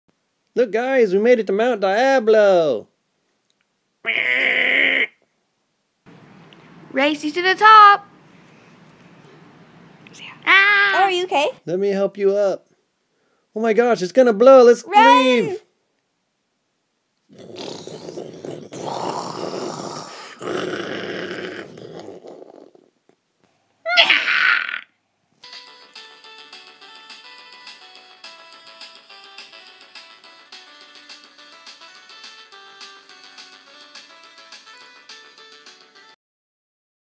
Stop Motion Ponies- The Volcano
Stop Motion video with ponies from My Little Pony